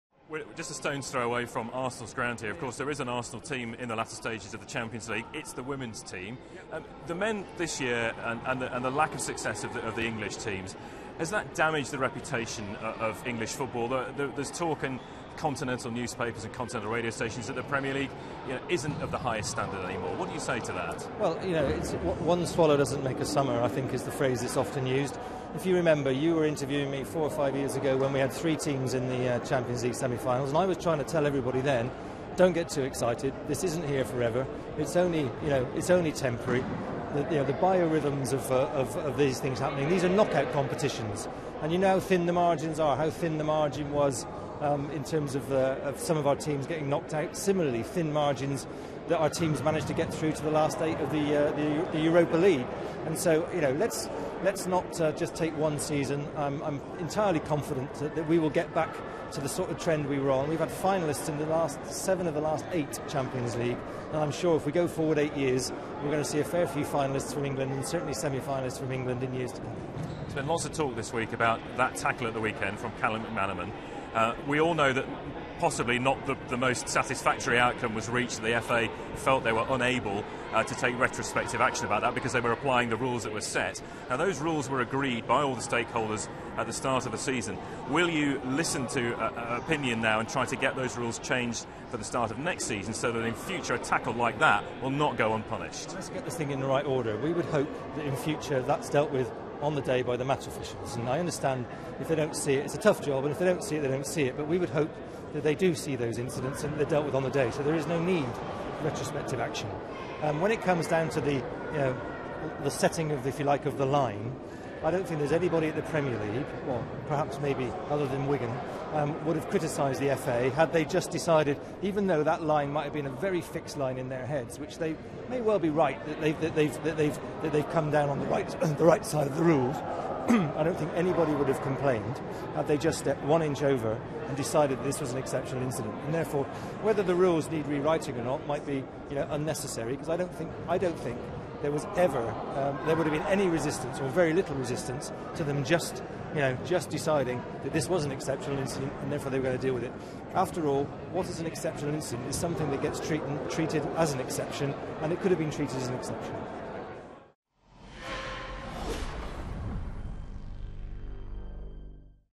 NEWS Sport - No need to change rules after McManaman tackle - Scudamore
Premier League chief Richard Scudamore says there is "no need" for retrospective punishments in the Premier League, in the wake of Callum McManaman's controversial unpunished challenge on Newcastle's Massadio Haidara.

Scudamore tells BBC Sport's Gordon Farquhar there would not have been any resistance to the FA declaring the challenge an "exceptional incident" - the only circumstances in which it is permitted to hand out retrospective punishment for an incident seen at the time by one or more officials - and charging McManaman.